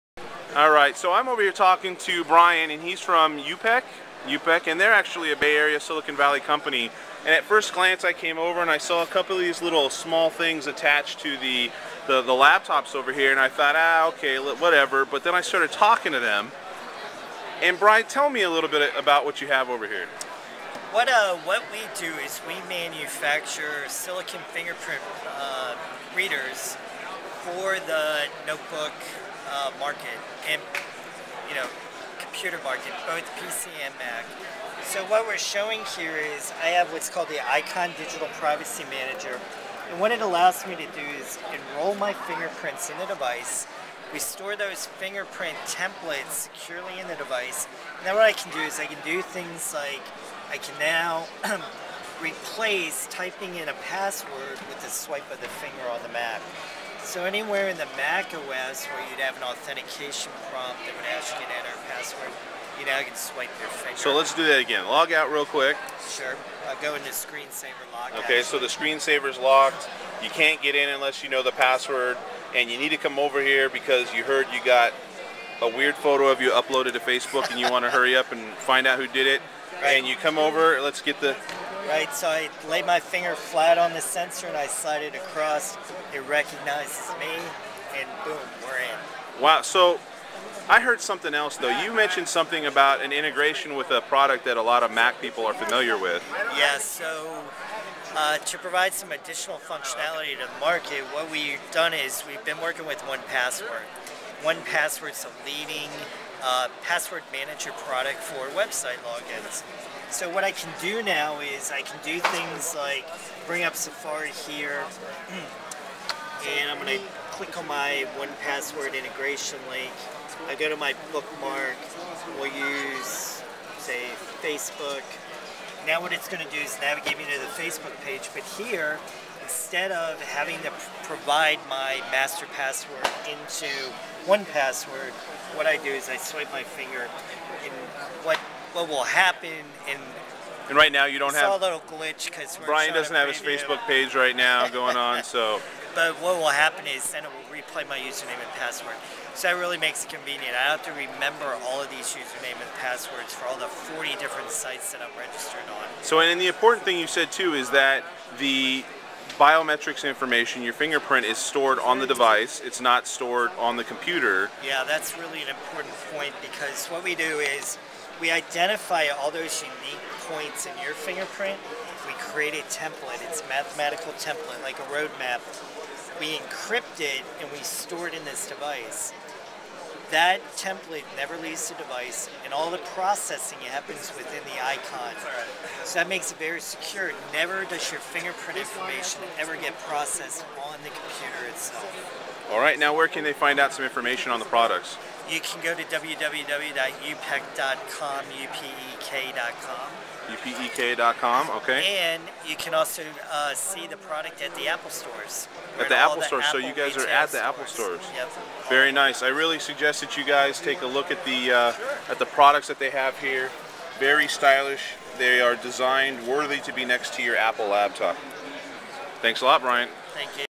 Eikon Digital Privacy manager by Upek at Macworld 2009
I got a personal demo of Upek's the Eikon biometric fingerprint reader for the Mac & PC. Now you don't need to buy a PC with one built in. They also announce a future integration with 1Password. Imagine logging into Facebook, Twitter, or online banking by just swiping your fingers. This was recorded for notebooks.com.